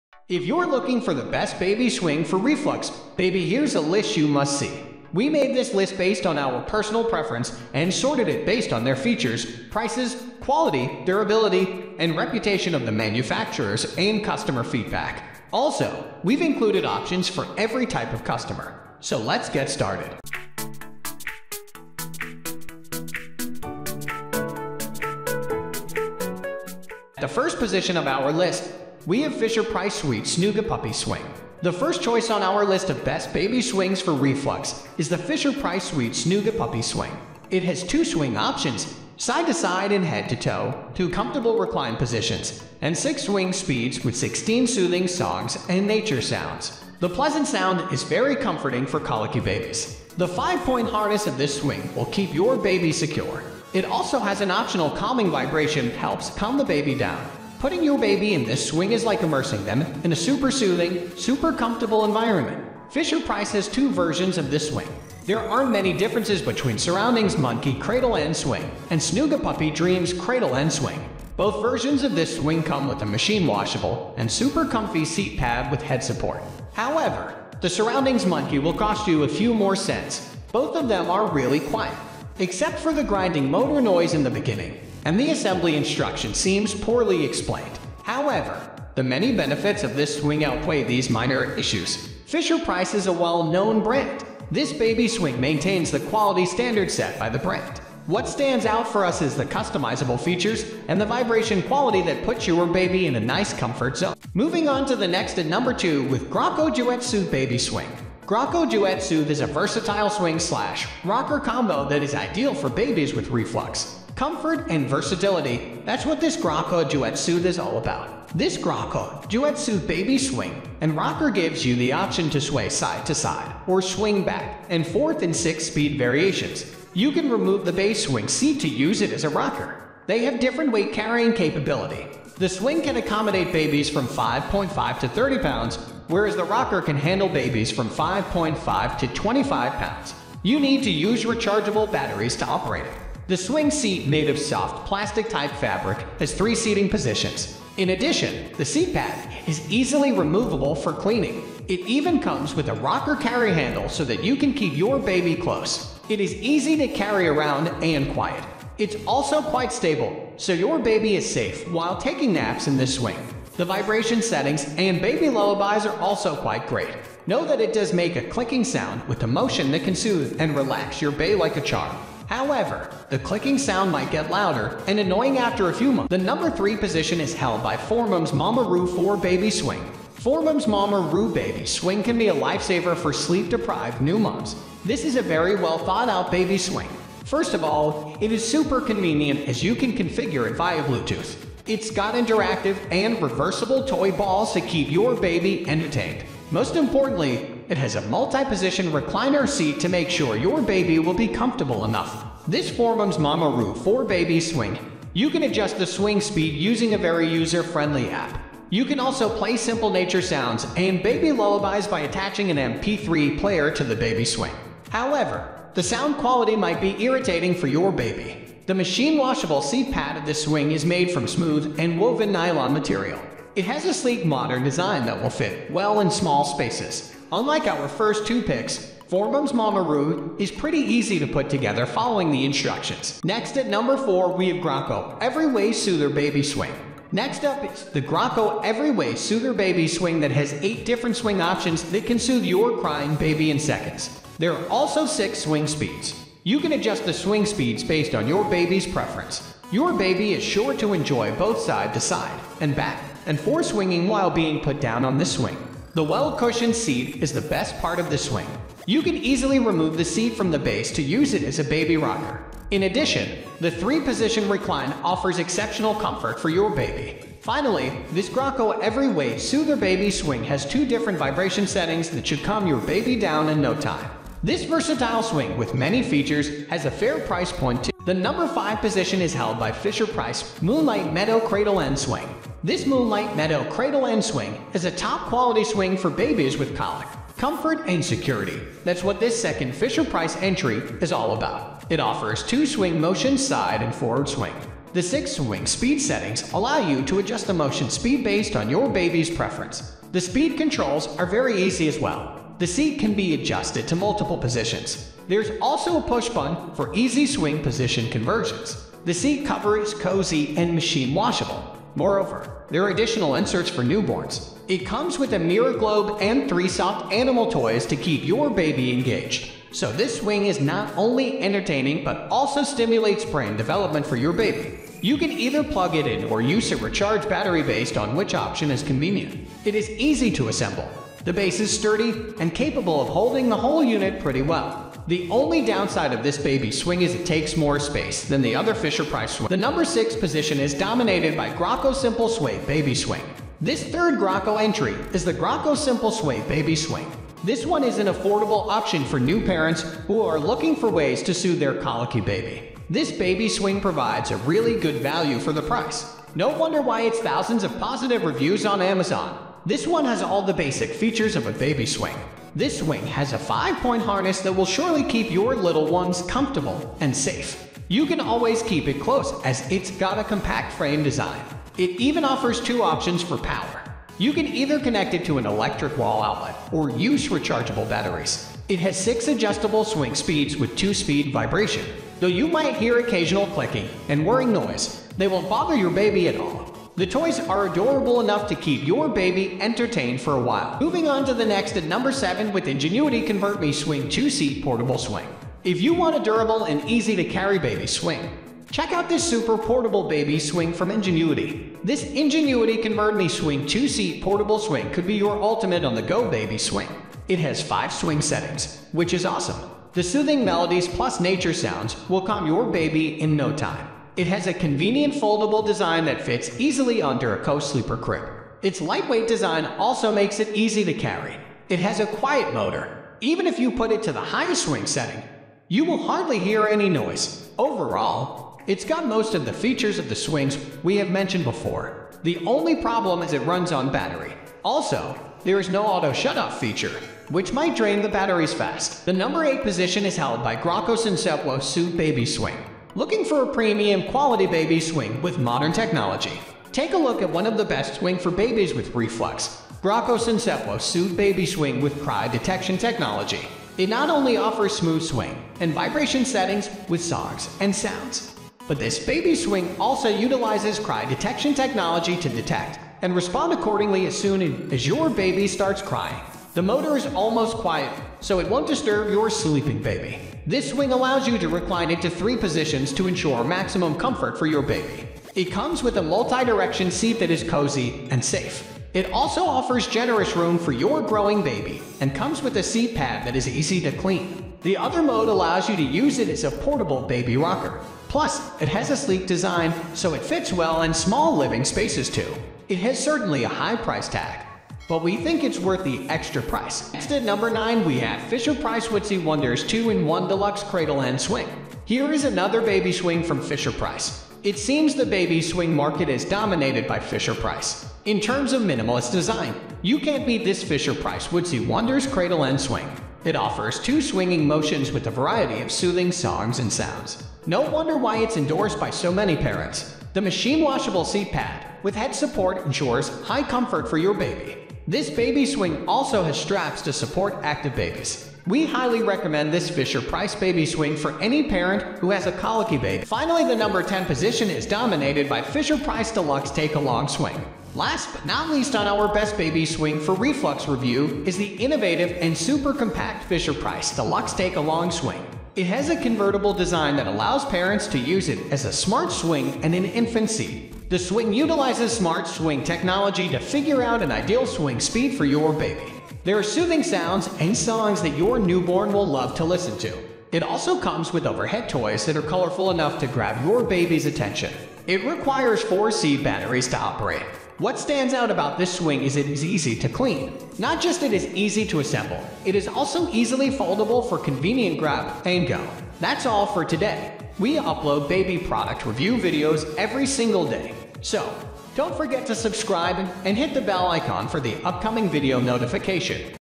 Best Baby Swing In 2022 - Top 10 Baby Swings Review
Links to the Best Baby Swing we listed in today's Baby Swing Review video & Buying Guide:

Top Rated Baby Swing List :

1 . Fisher-Price Sweet Snugapuppy Swing
2 . Graco DuetSoothe Baby Swing
3 . 4moms MamaRoo 4 Baby Swing
4 . Graco EveryWay Soother Baby Swing
5 . Fisher-Price Moonlight Meadow Cradle ‘n Swing
6 . Graco Simple Sway Baby Swing
7 . Ingenuity ConvertMe Swing-2-Seat Portable Swing
8 . Graco Sense2Soothe Baby Swing
9 . Fisher-Price Woodsy Wonders 2-in-1 Deluxe Cradle ‘n Swing
10 . Fisher-Price Deluxe Take-Along Swing

In this video, we have gathered the Baby Swing for your beloved baby. With all types of consumers in mind, we made this list through extensive research, pricing, quality, durability, brand reputation, and a few other crucial factors. Meaning, you can get the listed products with utmost confidence.